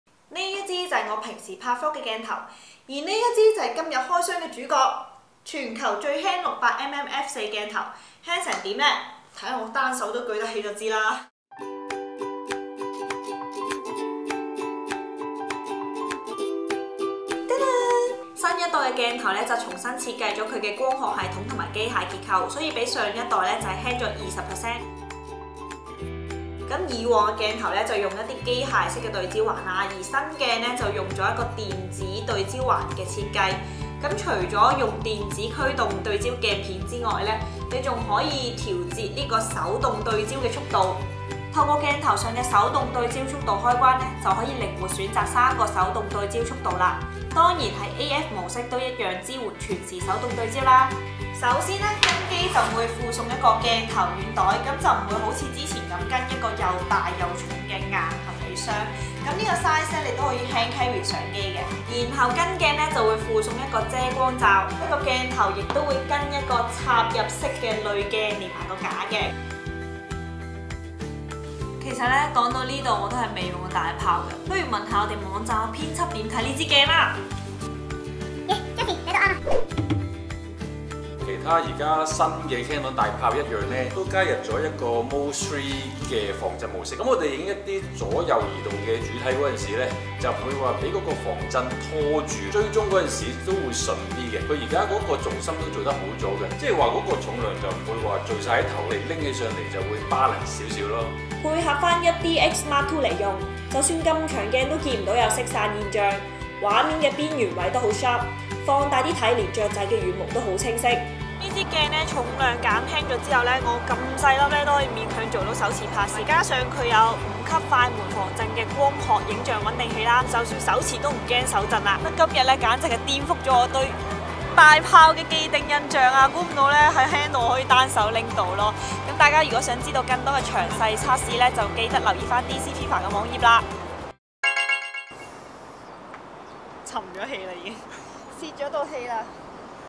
【開箱世界】全球最輕Canon EF 600mm f/4L IS III USM
新一代600mm大炮輕到可以手持！但其實我都要谷住道氣先hold到一分鐘，冇咩事我都係落返腳架先，話晒支鏡都價值十萬．．．

其實輕咗就唔單止為手持，有時攞構圖都易啲，仲可以調較3個手動對焦速度，想對快定對慢都有得set，加上高達5級快門防震​，唔知玩開大炮嘅師兄又收唔收貨呢？同期仲出埋 EF 400mm f/2.8L，賣點一樣係輕，不過越輕越貴是常識吧。